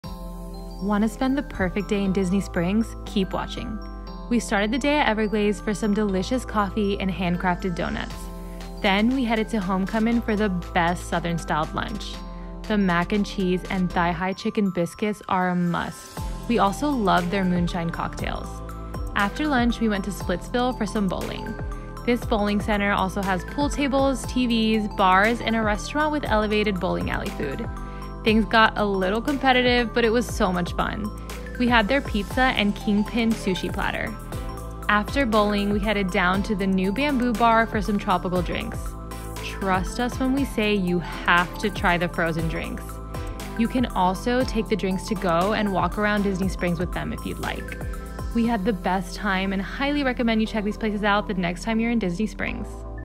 A day in Disney Springs | stuff to do in Orlando Florida
Let us show you what a day at Disney Springs with us looks like!

UNATION is your personal guide to local events in Orlando. Find the best stuff to do and discover great events around you!